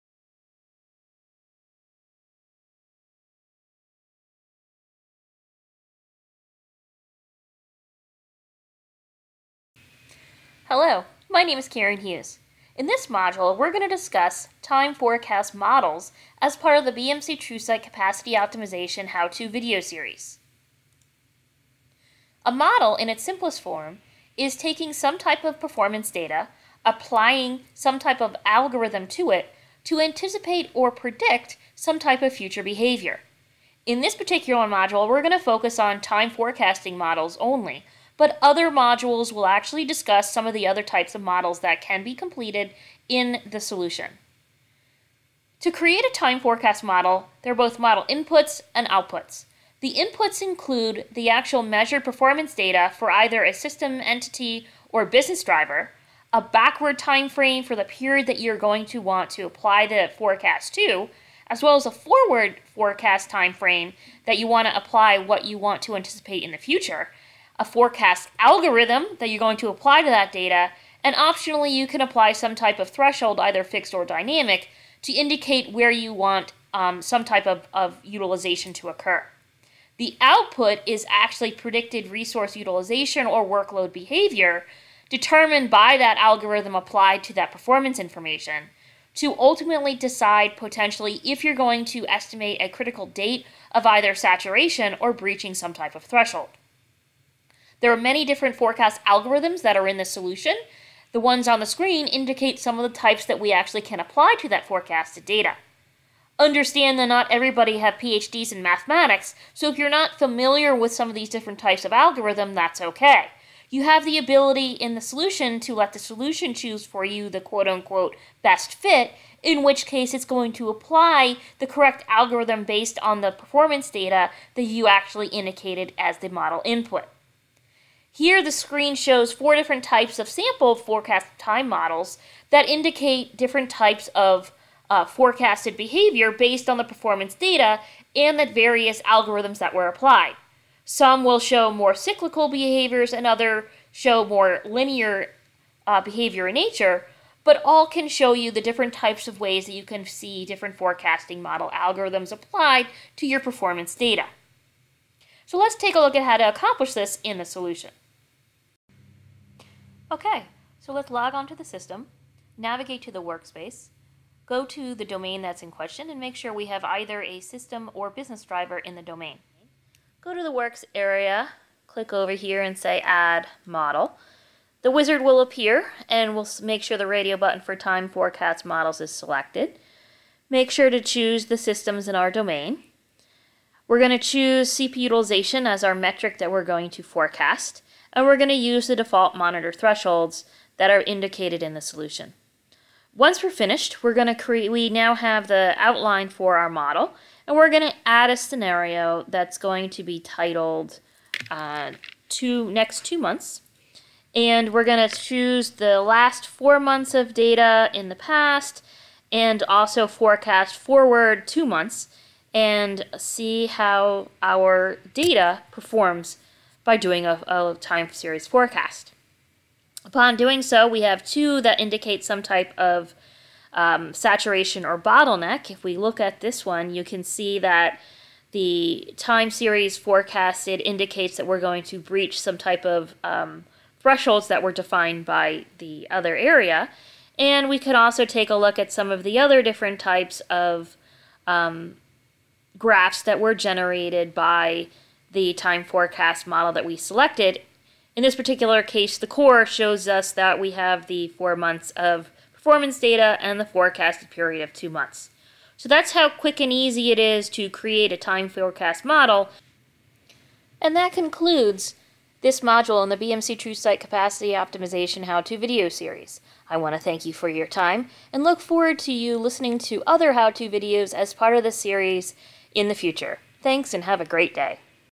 Creating a Time Forecasting Model | TrueSight Capacity Optimization
This video will show how to create a Time Forecast Model in BMC TrueSight Capacity Optimization as part of the BMC TrueSight Capacity Optimization How To video series.